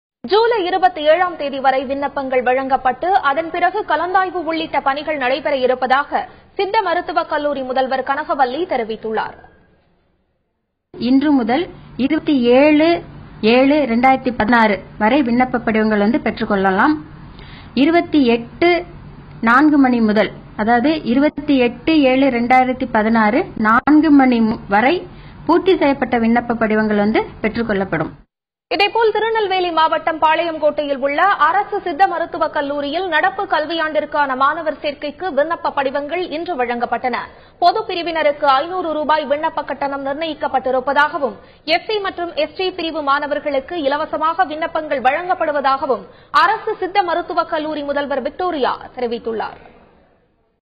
Applications for Siddha Medicine to be Issued till July 27 - Kanakavalli, Cheif | Thanthi TV
Students can obtain Applications for Siddha Medicine till July 27 - Kanakavalli, Cheif | Thanthi TV